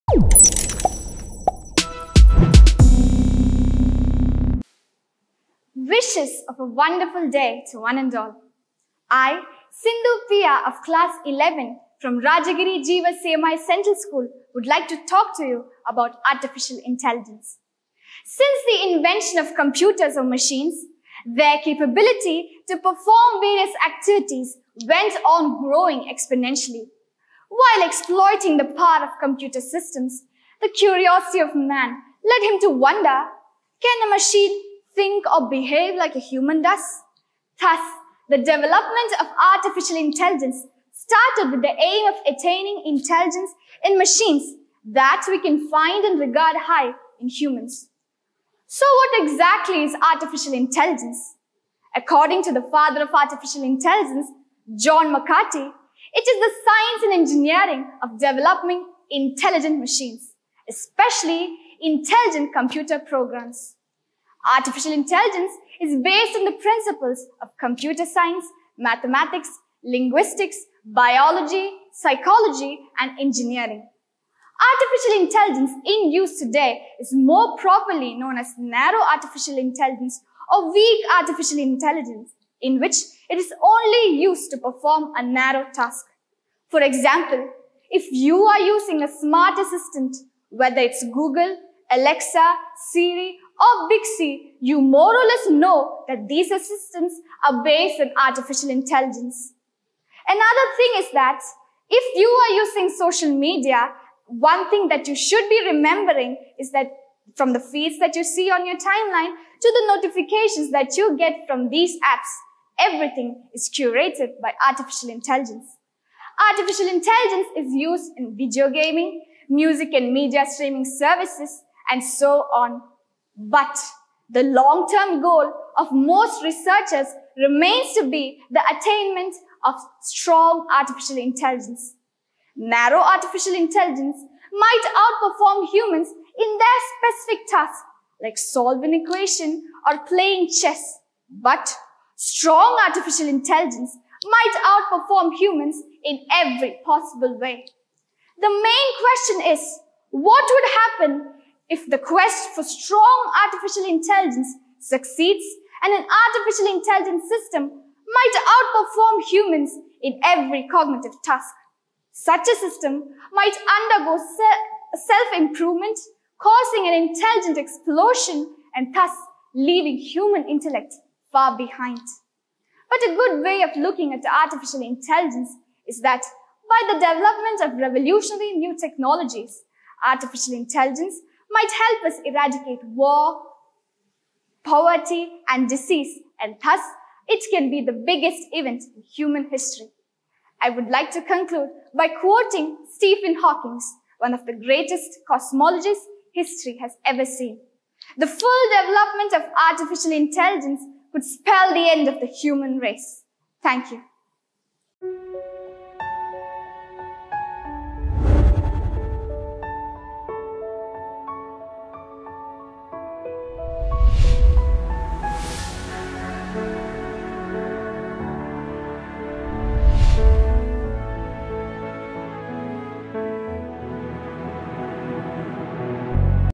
How Artificial Intelligence Will Change Our World? Sindhu P R | Jeevas CMI Central School, Aluva
PRIME TALKER:  SINDHU PR
SCHOOL: JEEVASS CMI CENTRAL SCHOOL, ALUVA